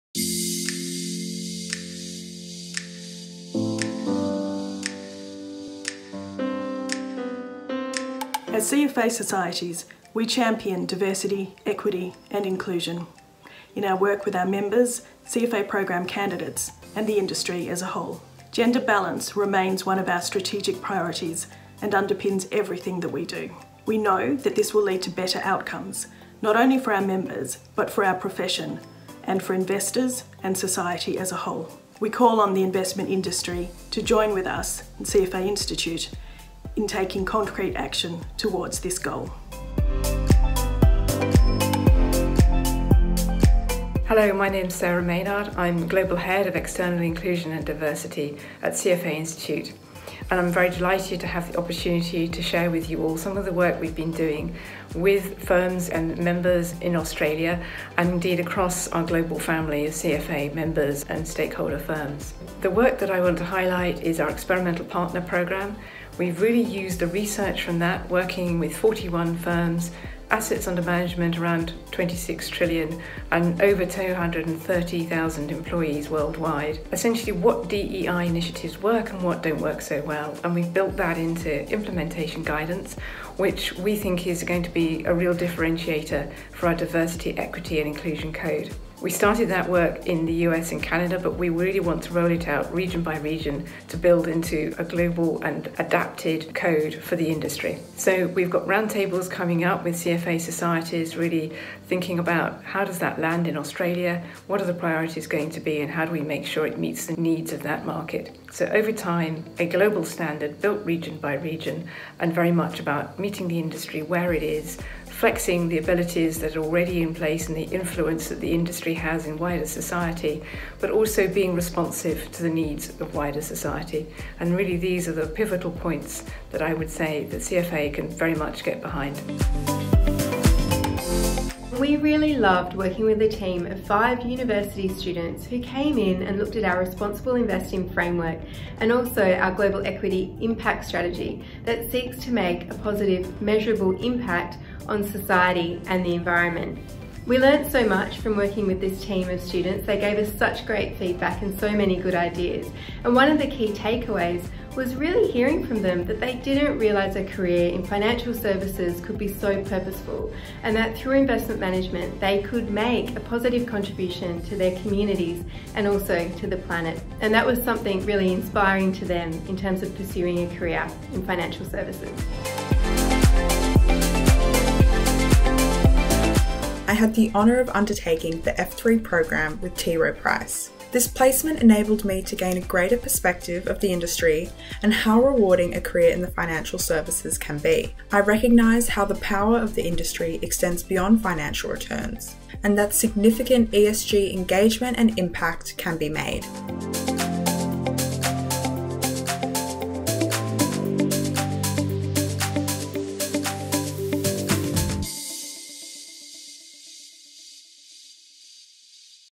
Diversity, Equity and Inclusion
At CFA Societies, we campion Diversity, Equity and Inclusion, in our work with our members, CFA Program candidates and the industry as a whole.

Gender balance remains one of our strategic priorities and underpins everything that we do. We know that this will lead to better outcomes, not only for our members but for our profession, and for investors and society as a whole. We call on the investment industry to join with us, at CFA Institute, in taking concrete action towards this goal.